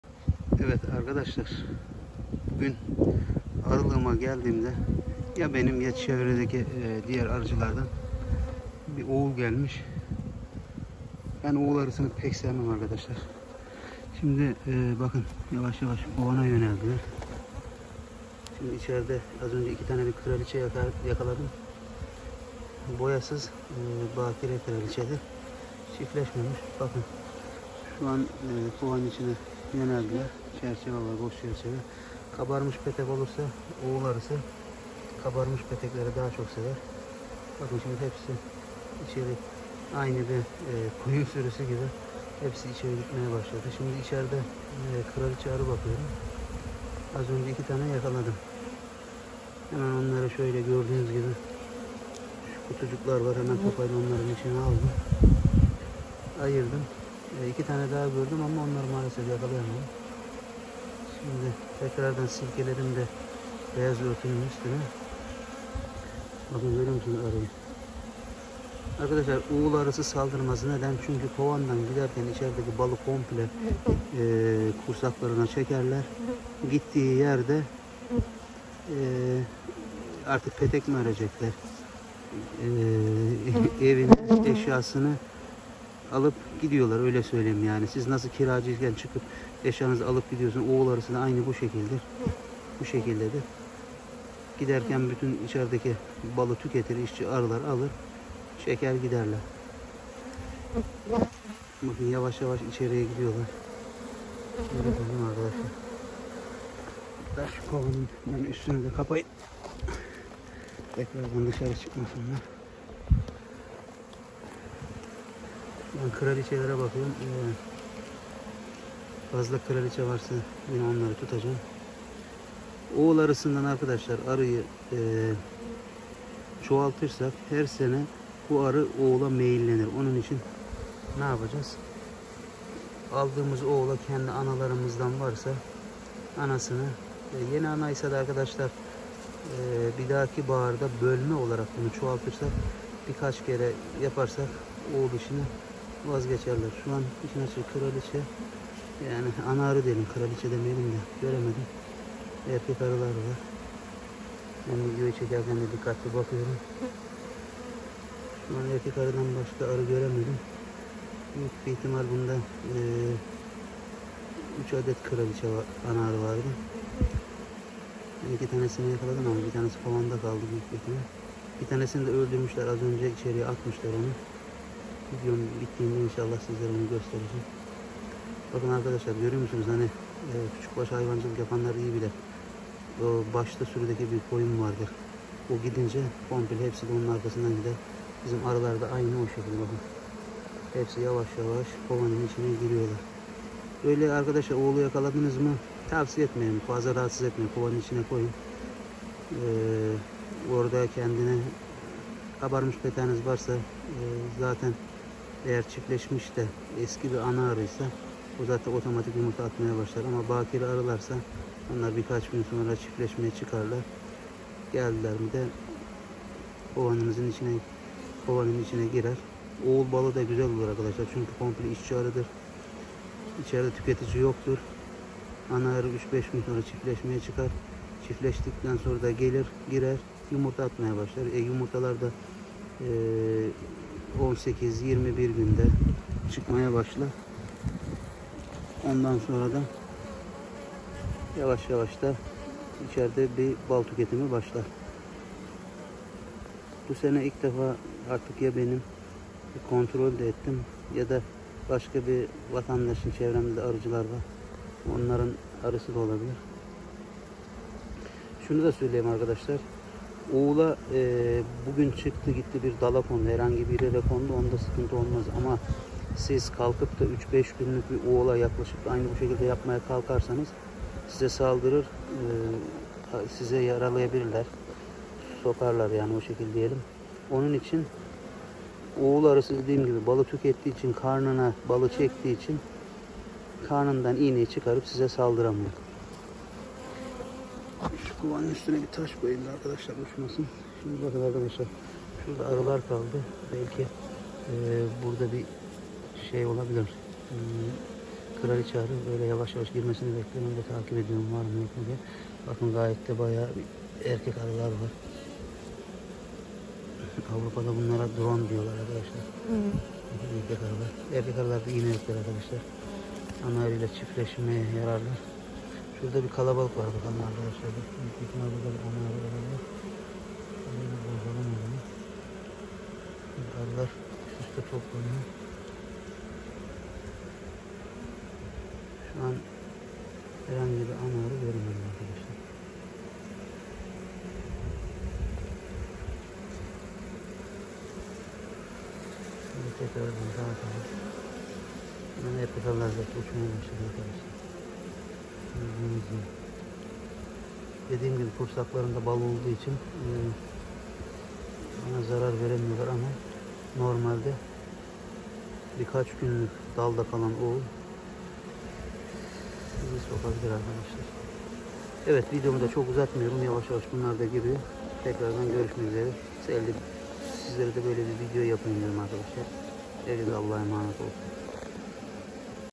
OĞUL ARISI
Ayrıldığımıza gelen oğul nasıl kovana alınır